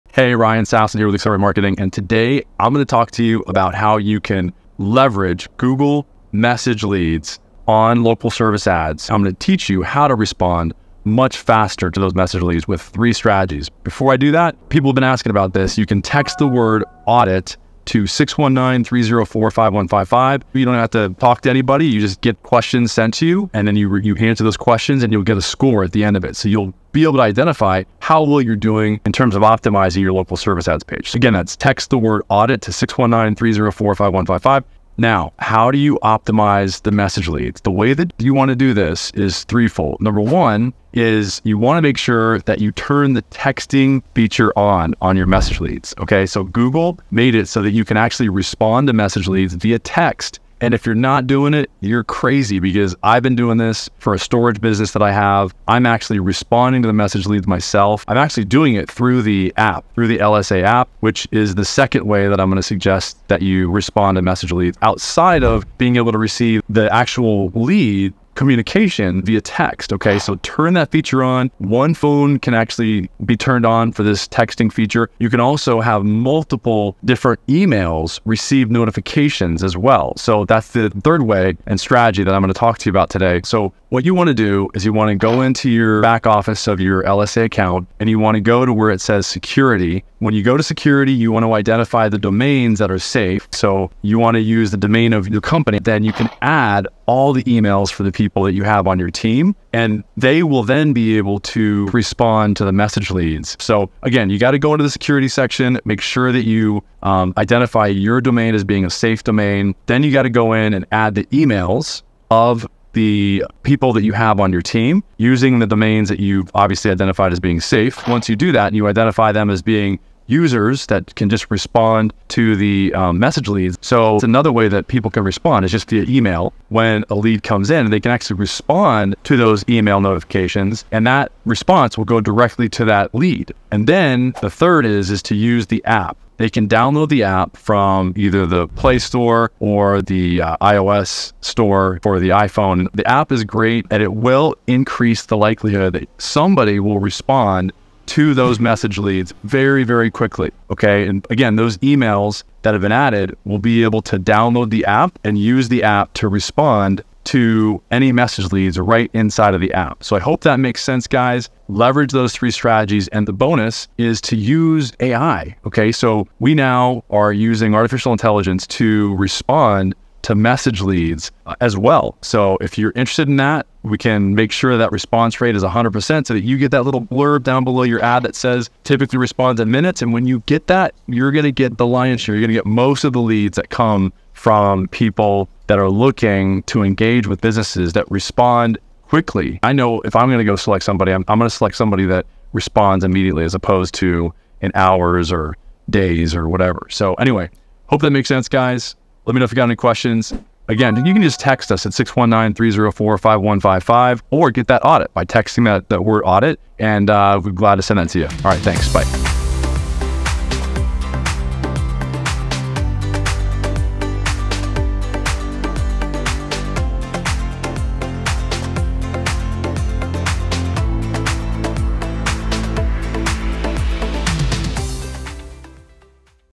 Optimize Local Service Ads with Message Leads: Ways to Improve LSA Performance
Ryan Steinolfson from Accelerate Marketing talks message leads, the newest feature from Google and Local Service Ads. learn about how to use all of the LSA tools available, including the LSA app and text messages, to improve your Local Service Ads performance.

Chapters:
00:00 Intro
00:45 Message Clients: New Text Feature
01:02 Google LSA App
01:32 Tutorial: Add Emails to Google LSA Notifications
02:17 Use the Local Service Ads App
02:50 Using AI as a Tool